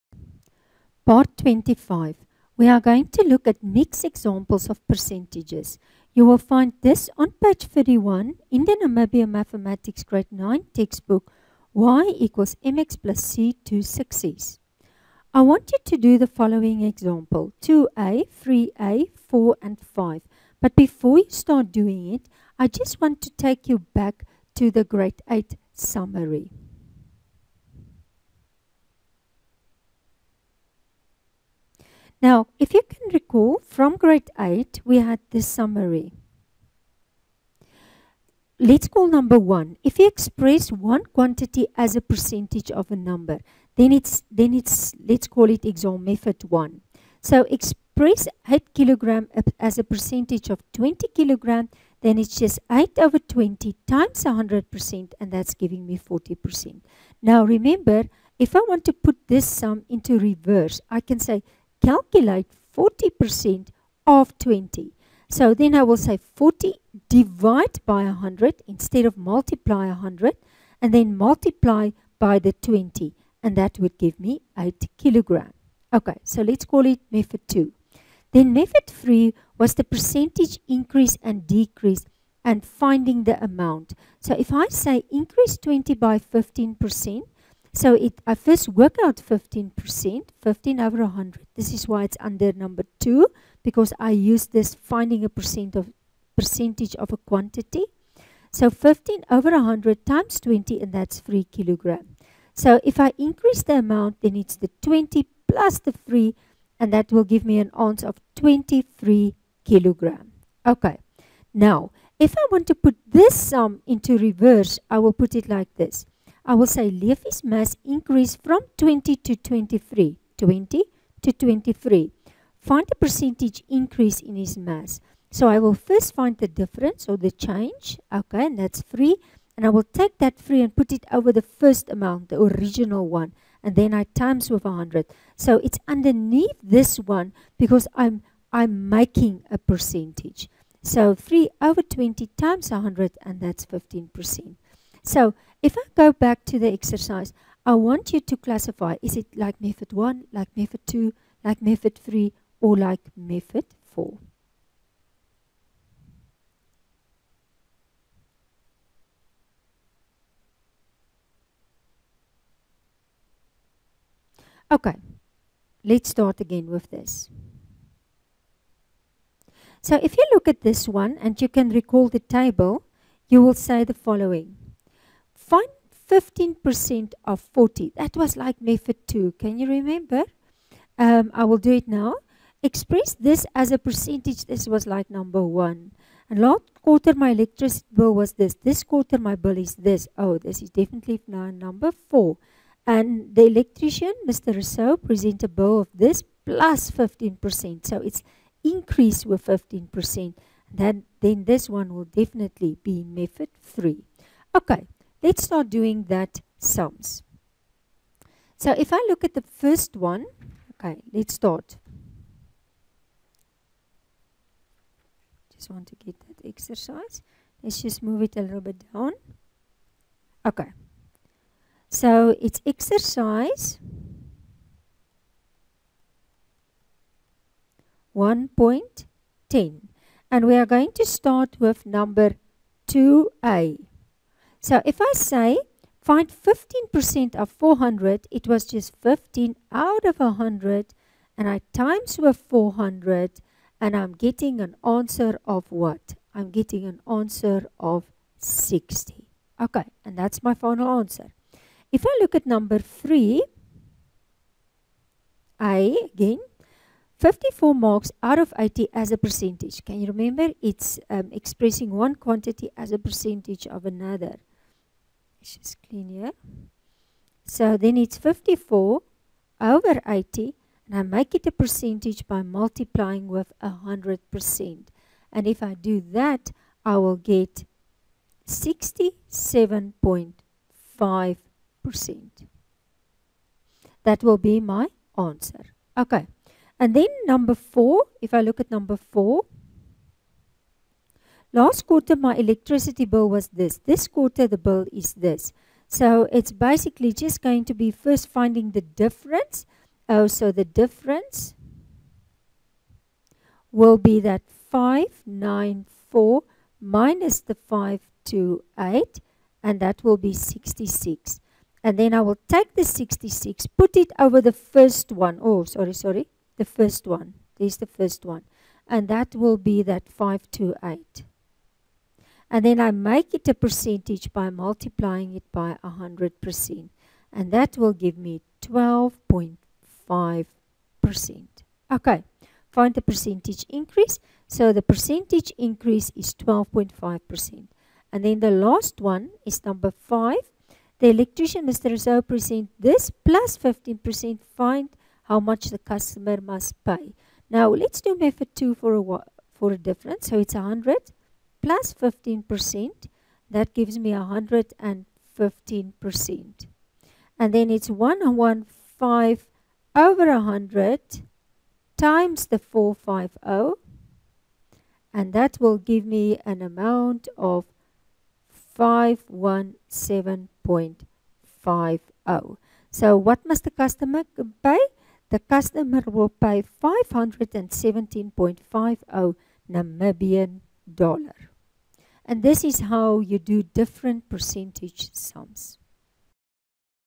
1.25 Mixed Examples of Percentages - Chapter 1 - part 25
Mixed Examples of Percentages. This can be found in the Namibian grade 9 mathematics textbook "Y=mx+c to Success".